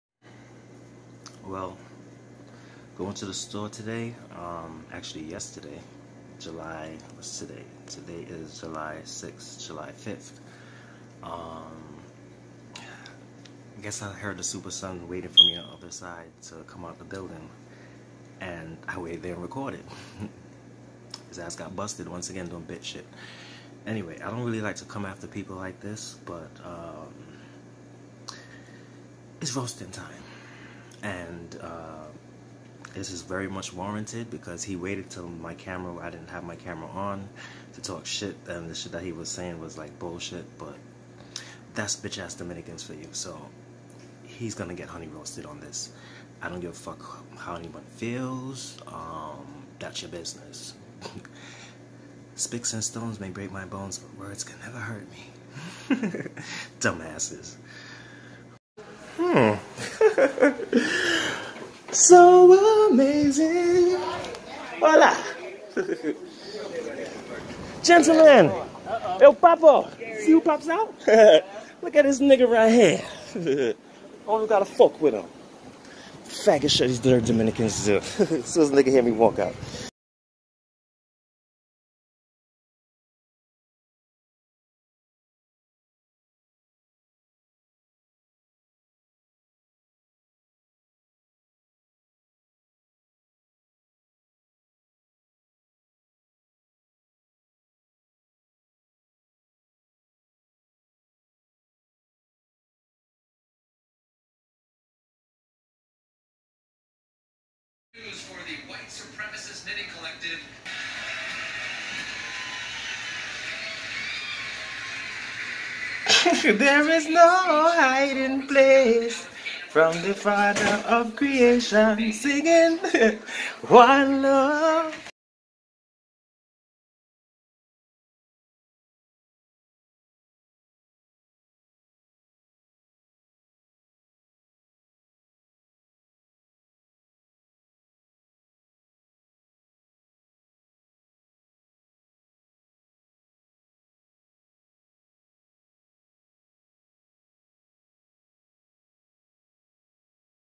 Supers son waiting for me to leave the building little did he expect 🎥
Lmao this is the Typical Dominican broad in the shoulders and got the fattest vaginas 😹🤔

Too bad his brother had to learn the hard way.
Intimidation doesn't work well for me i don't care how big you are.
Be a gangster for the camera 🎥😂.

His mother got more testicles and testosterone than he does.

His sister could probably throw down with all that 🇩🇴🍰🍑👀😂

I don't come after females just muscle bound idiot's like this 😂.

If i was to be really wicked i would record his sister's little village of children walking in and out of the Crack House / Crazy Farm 👍🏽.

Just remember (Karma is a Bitch)😹